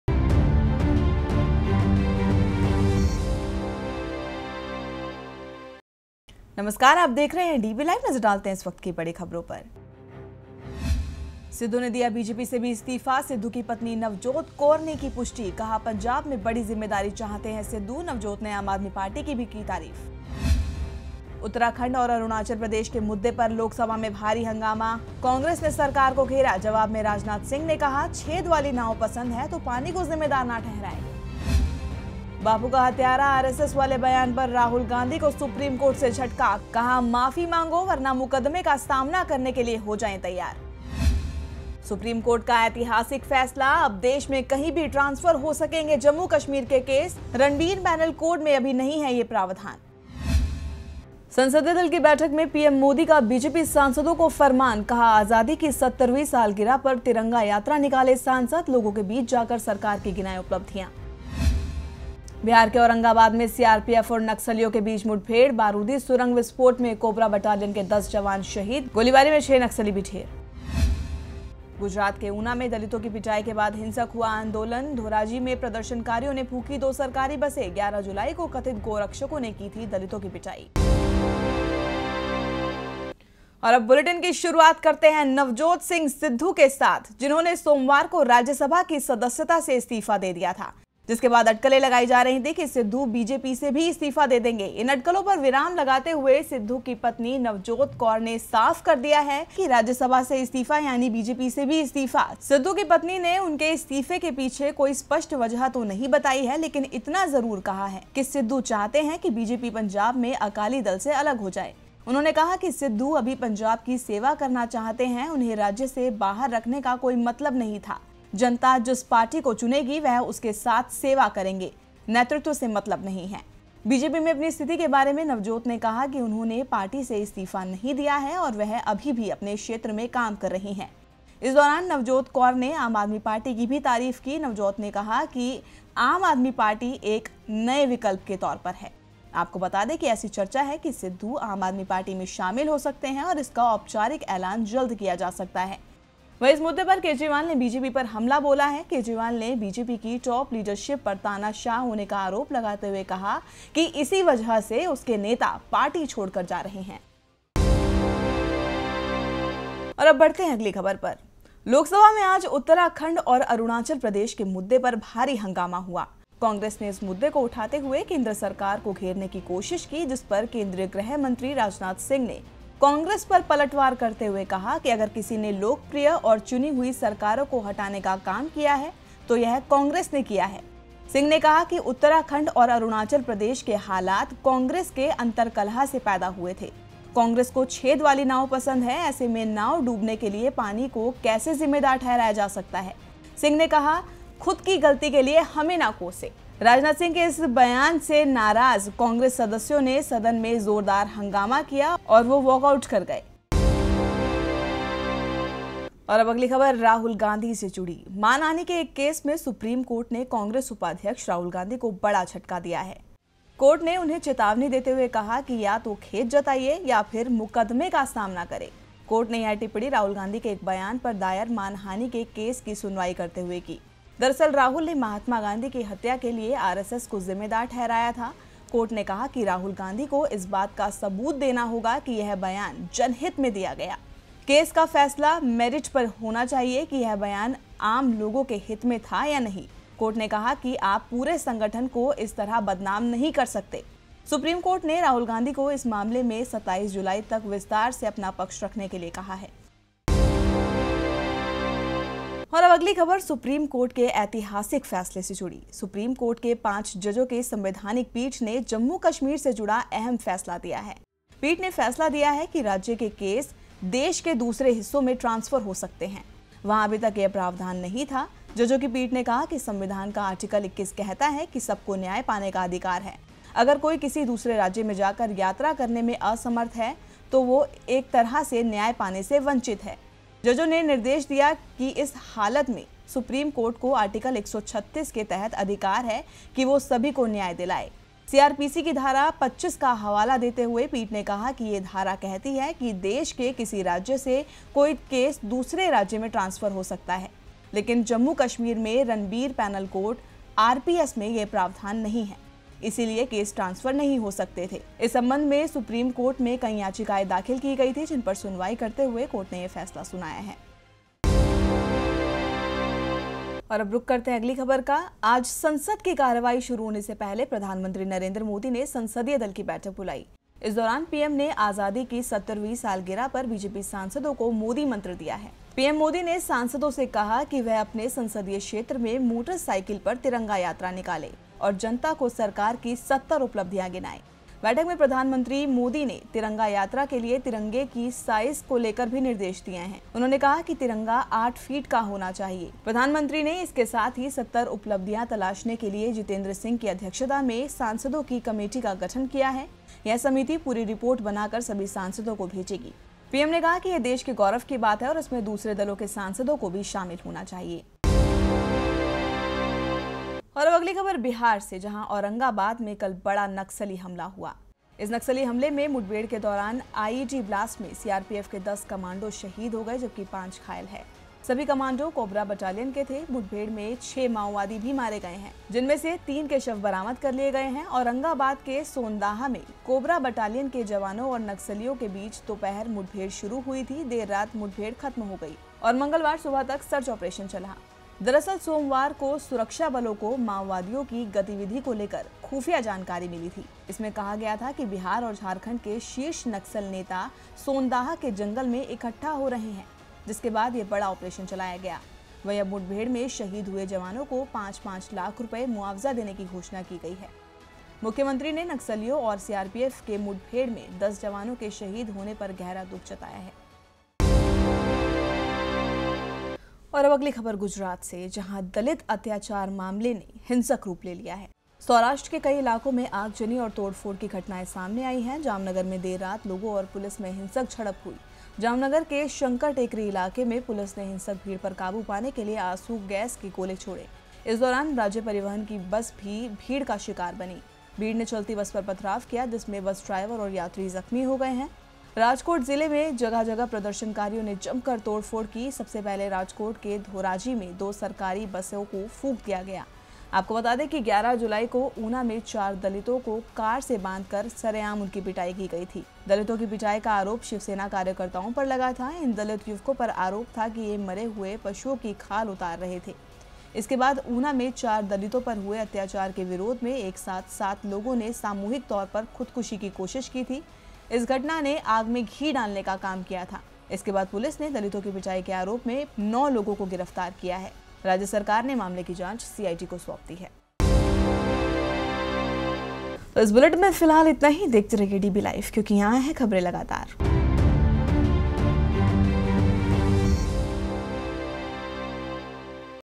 DB LIVE | 19 JULY 2016 | NEWS BULLETIN
1..सिद्धू ने दिया बीजेपी से भी इस्तीफा...सिद्धू की पत्नी नवजोत कौर ने की पुष्टि...कहा, पंजाब में बड़ी ज़िम्मेदारी चाहते हैं सिद्धू...नवजोत से आम आदमी पार्टी की भी तारीफ की।

2.. उत्तराखंड और अरूणाचल प्रदेश के मुद्दे पर लोकसभा में भारी हंगामा...कांग्रेस ने सरकार को घेरा..जवाब में राजनाथ सिंह ने कहा, छेद वाली नांव पसंद है तो पानी को ज़िम्मेदार न ठहराएं।

3... 'बापू का हत्यारा RSS' वाले बयान पर राहुल गांधी को सुप्रीम कोर्ट से झटका. कहा- माफी मांगो वर्ना, मुकदमे का सामना करने के लिए हो जाएं तैयार।

4.. सुप्रीम कोर्ट का ऐतिहासिक फैसला... अब देश में कहीं भी ट्रांसफर हो सकेंगे जम्मू-कश्मीर के केस... रनबीर पैनल कोड में अभी नहीं है ये प्रावधान

5...संसदीय दल की बैठक में पीएम मोदी का बीजेपी सांसदों को फरमान...कहा, आजादी की 70वीं सालगिरह पर 'तिरंगा यात्रा' निकालें सांसद...लोगों के बीच जाकर सरकार को गिनाएं उपलब्धि‍यां